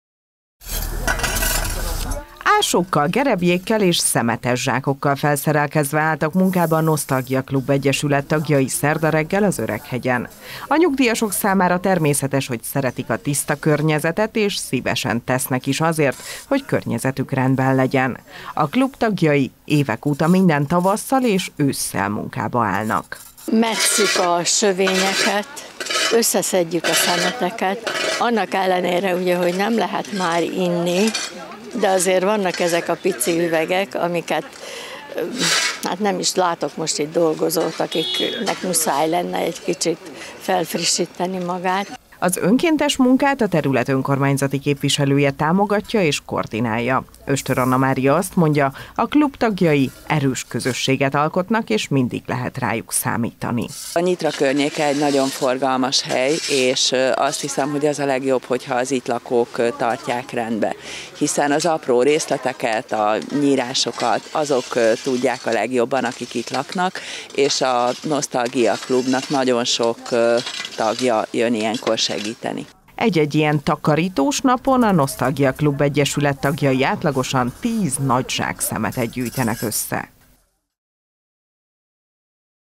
Megszépítették a környezetüket
Saját környezetüket szépítette meg a Székesfehérvári Nosztalgia Klub Egyesület. A szervezet tagjai számára hagyomány, hogy minden tavasszal és ősszel rendbe teszik a Nyitra ABC környékét.